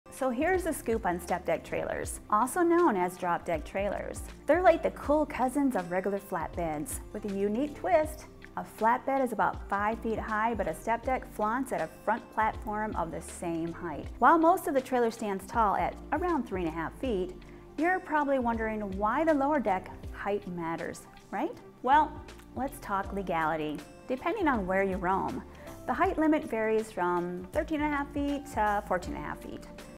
What Is A Step-Deck Trailer?
🚛 Curious about step-deck trailers? Look no further! In this video, we dive into the world of step-deck trailers, explaining what they are and why they're essential in the world of transportation.

📋 Step-deck trailers, also known as drop-deck or lowboy trailers, are specialized transportation solutions with a unique design. Their lower deck and raised front section make them perfect for transporting tall or oversized cargo, such as heavy machinery, construction equipment, and more.

🛠️ Learn about the advantages of using step-deck trailers, including increased cargo height clearance, improved stability, and ease of loading and unloading. Discover the types of cargo commonly transported on these trailers and the industries that rely on them.

💡 Whether you're a trucking enthusiast, a professional driver, or simply curious about transportation technology, this video provides valuable insights into step-deck trailers.